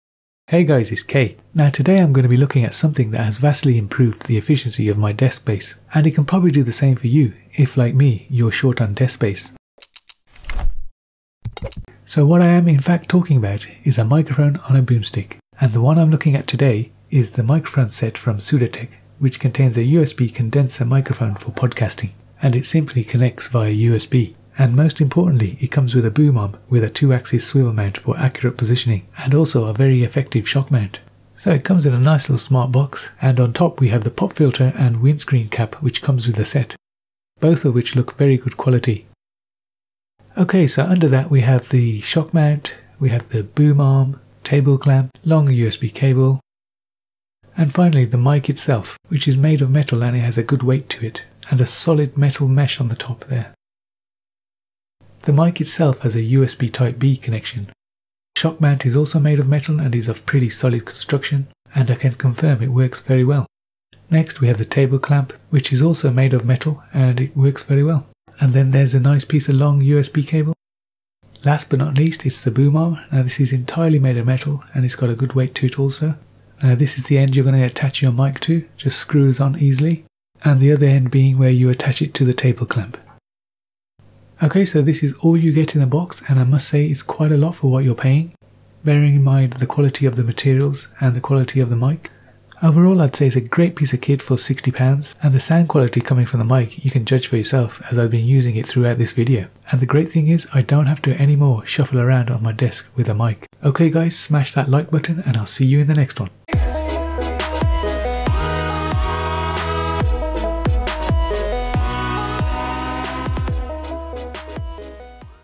USB Cardioid Condenser Mic Kit
This professional SUDOTACK mic kit has all you need to get started for creating Youtube videos and podcasts. It is simple to use and plugs straight into your pc or mac's usb port, with no drivers to download, just plug and play!

Perfect for a beginner! I'm fairly new to streaming and pc gaming and was looking for good quality mic on a budget and this has most definitely delivered, very nicely packaged, audio quality is clear and crisp, very sturdy, overall,  I cannot fault this mic!

PRIVACY POLICY and FULL DISCLOSURE:
°TechFigure is a participant in the Amazon Services LLC Associates Program (UK & US)
*All my opinions are my own.